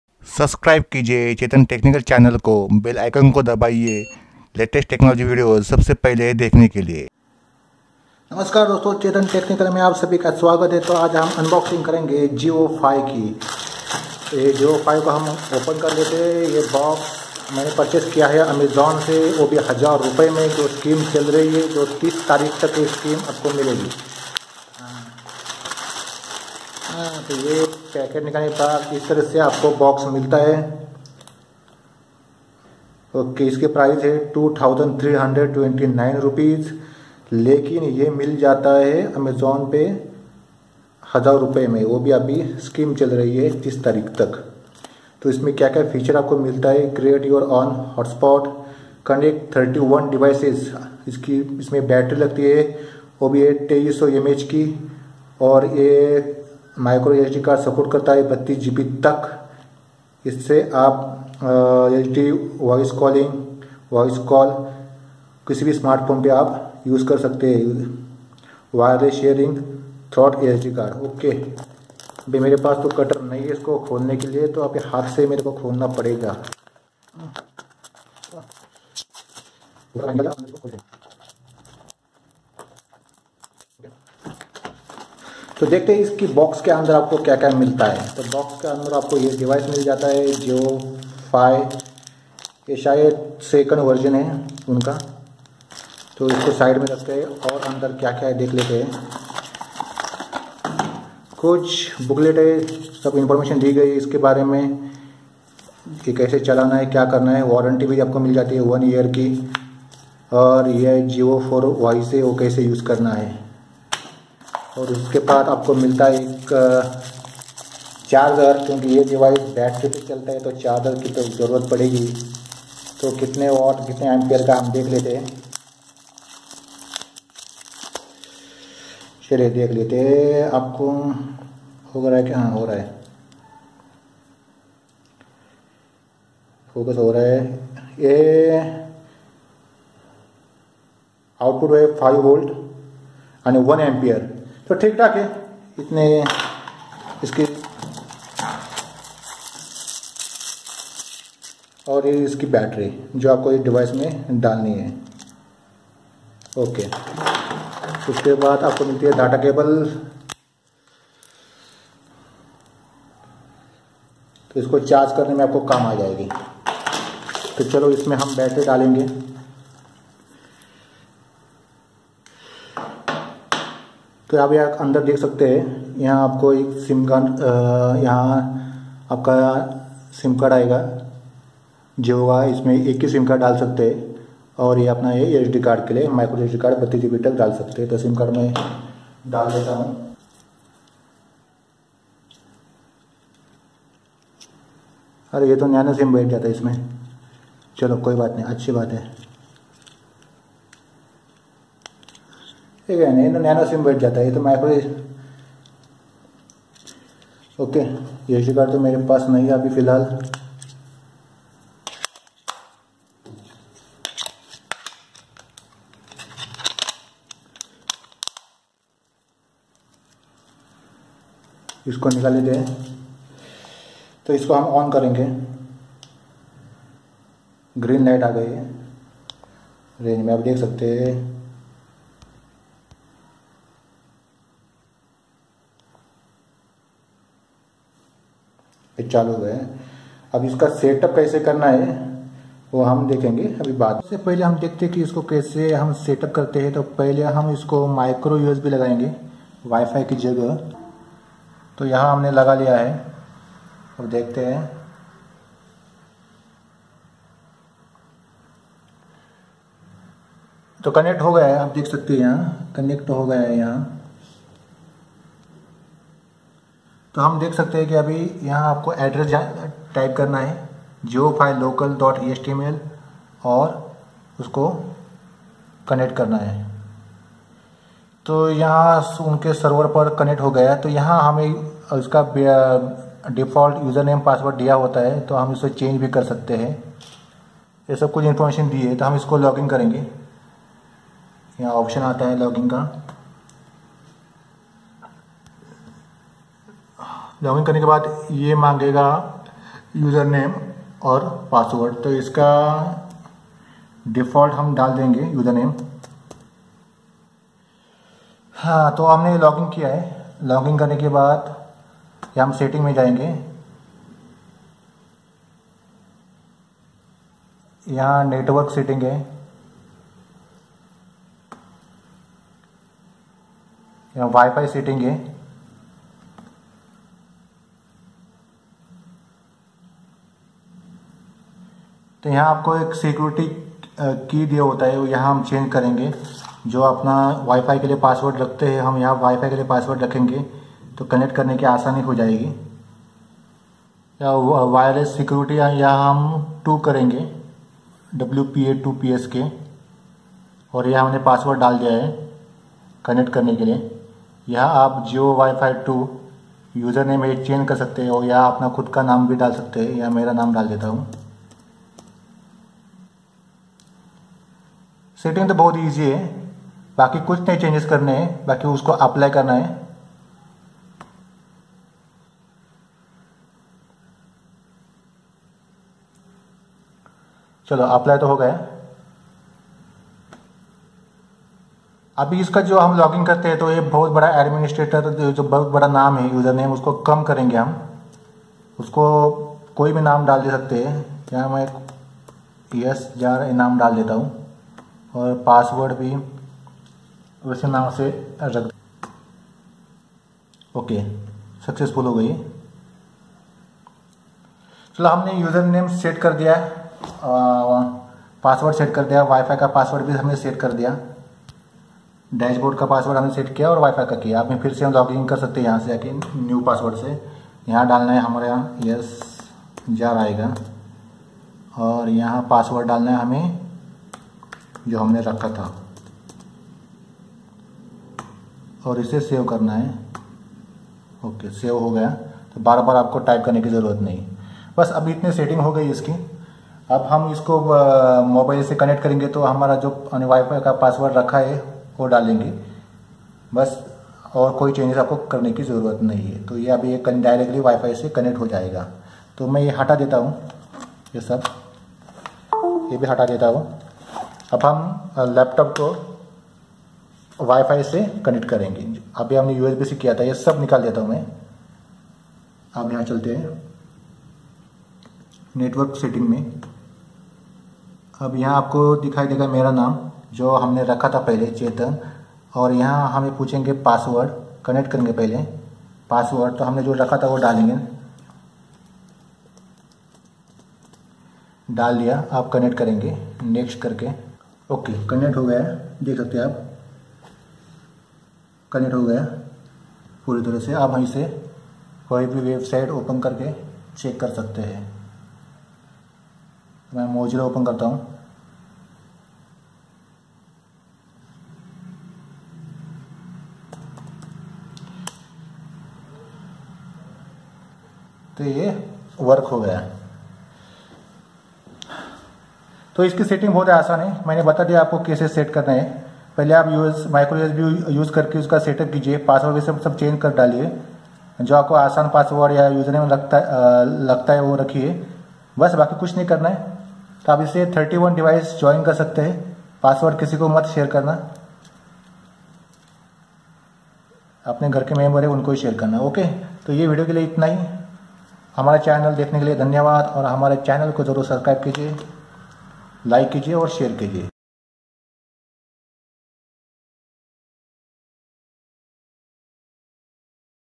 JioFi M2S Wireless 4G Portable Data - Unboxing & Set Up
jiofi 2 ki unboxing aur set up kaise karte hai. ye iss video me dikhaya gaya h.